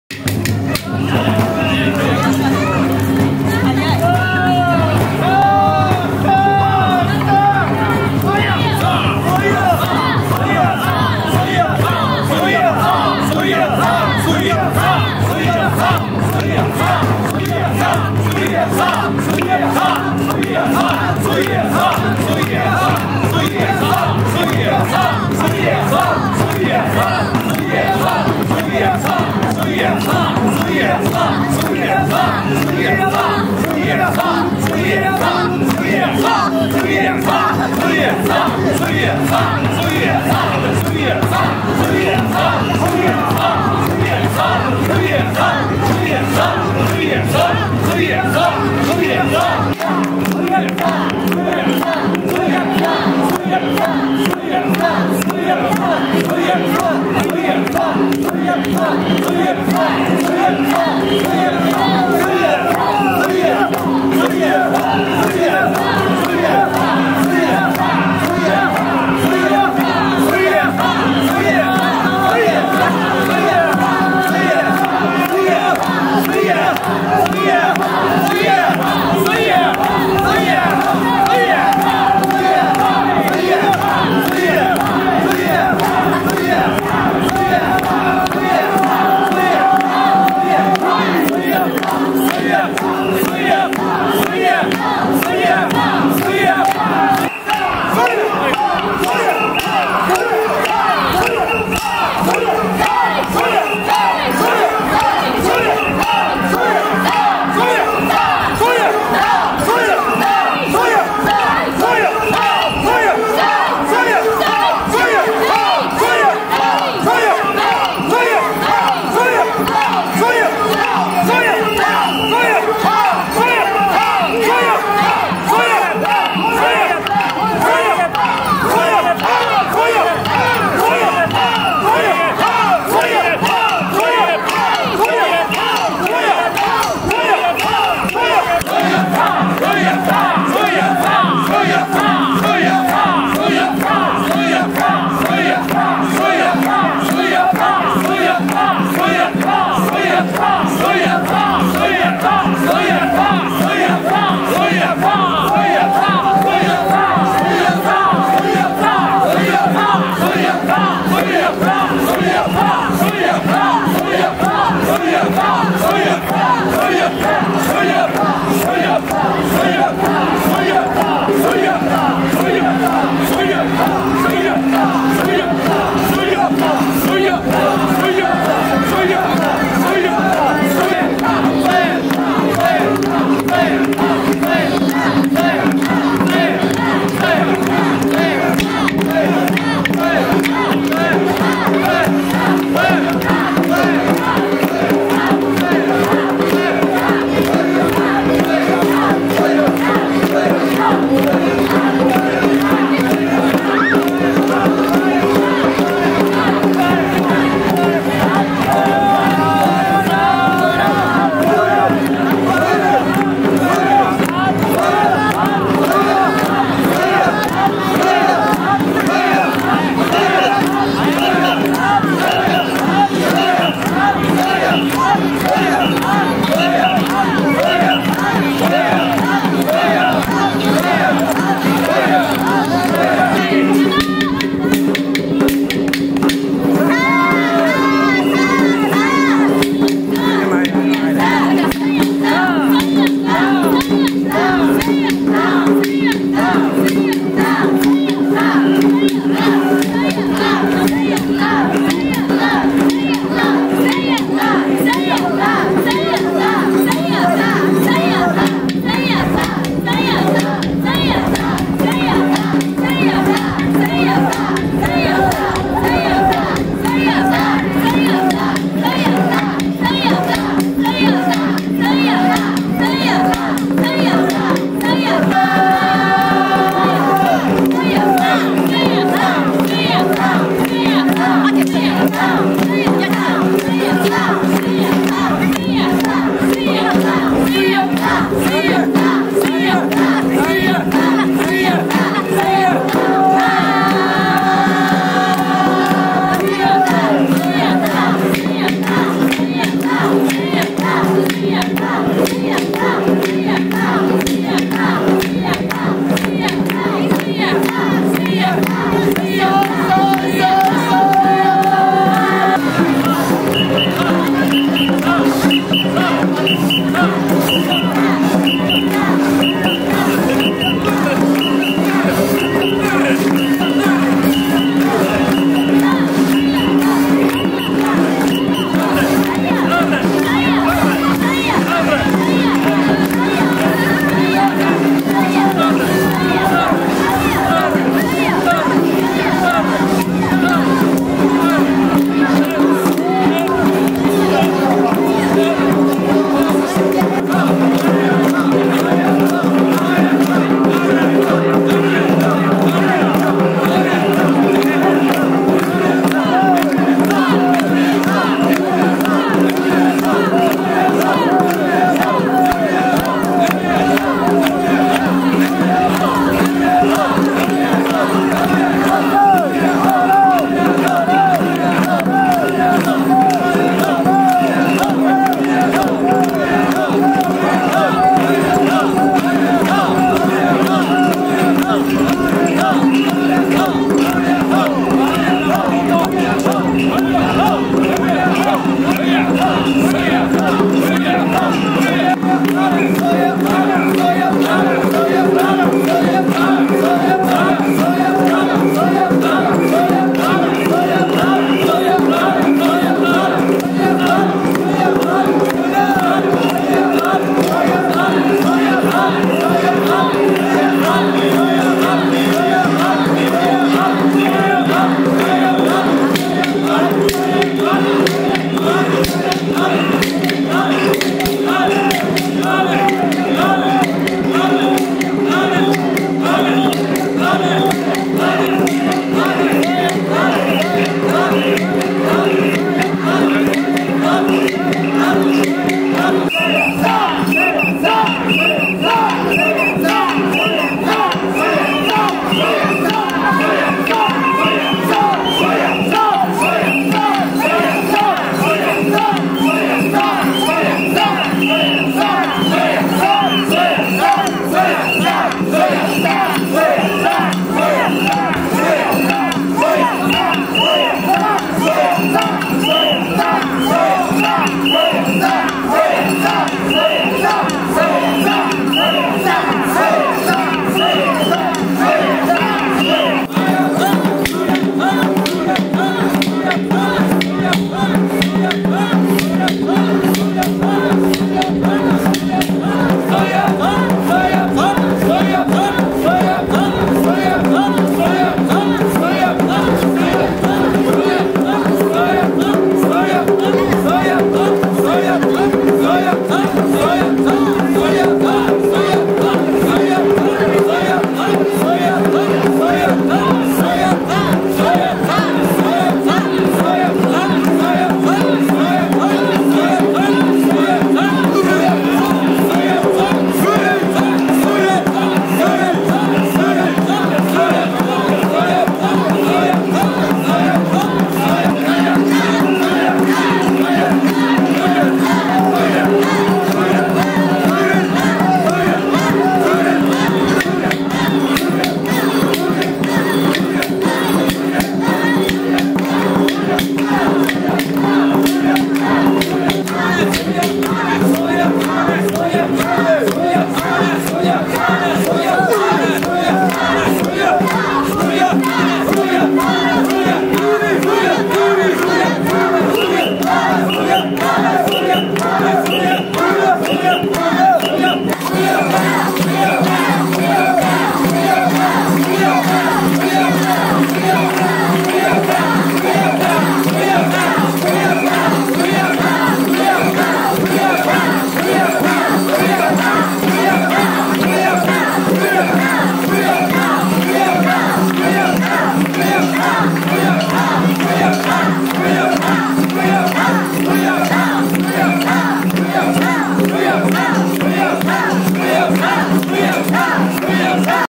甲子秋まつり2019 (その2)
2019年10月19日・20日に富士本町周辺で開催された「甲子秋まつり」の模様です。